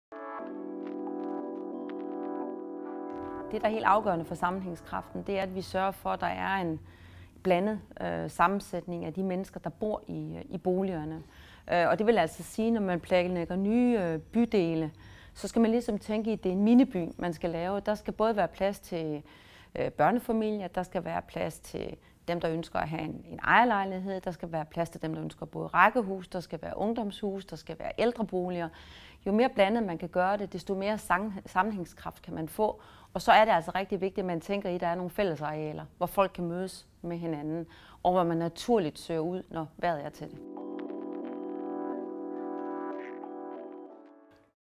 "Hvordan skaber vi sammenhægskraft?" v. Lene Espersen, adm. direktør i Danske Arkitektvirksomheder
Welcome Home - Åbningsudstilling af "udforsk fremtidens bolig" på Dansk Arkitekturcenter i BLOX. Udstillingen løber fra d. 7. maj til 23. september 2018.